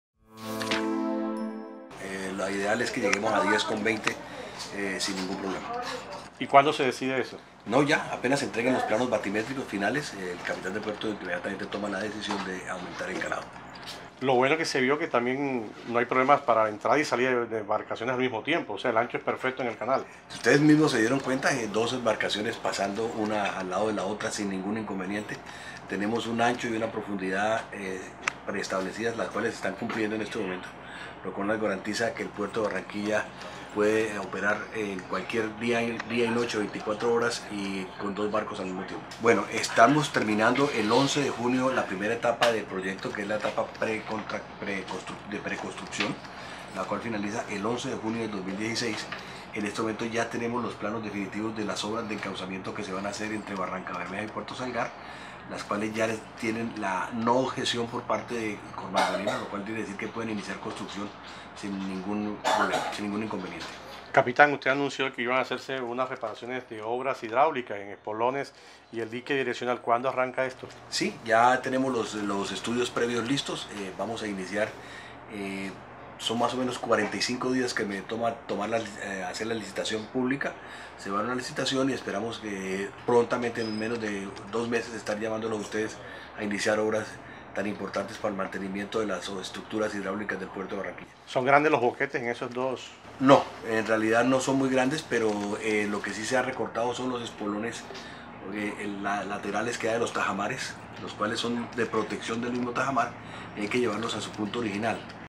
Dragado removió 1,2 millones de m3 de sedimento en el Río
Con los trabajos realizados por la draga podrán ingresar a la zona portuaria de Barranquilla buques de hasta 10,2 metros de calado.